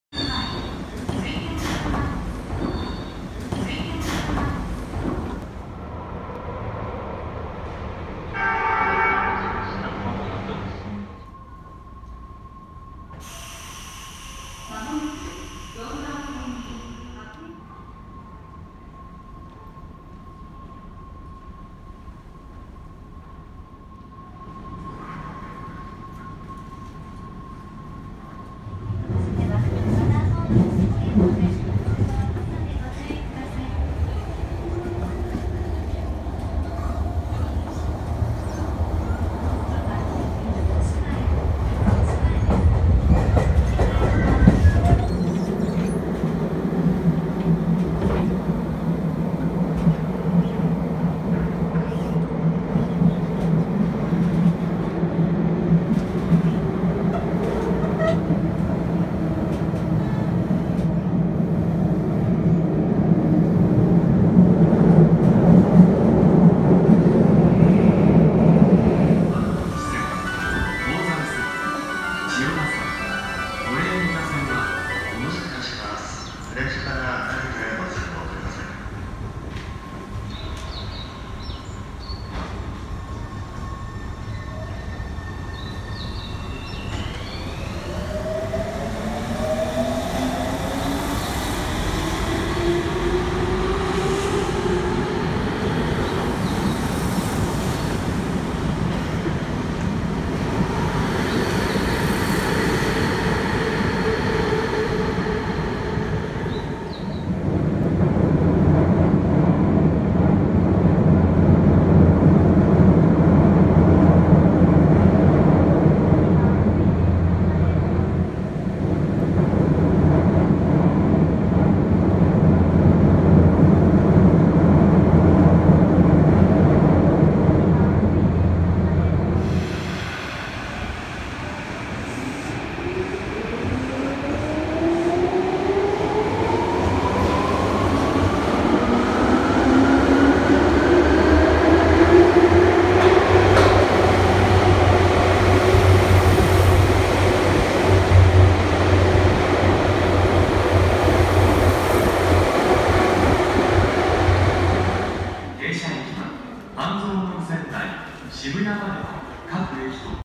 東京メトロ 半蔵門線内を走行する 東武30000系電車 と 東急8500系青帯電車 Bunkamura(文化村)号
4両➕6両の東武30000系 とてもスマートな車体が大好きです！
2018年4月22日から運行されている 東急8500系ラッピング車両 こちらは1編成 乗車出来てラッキーでした。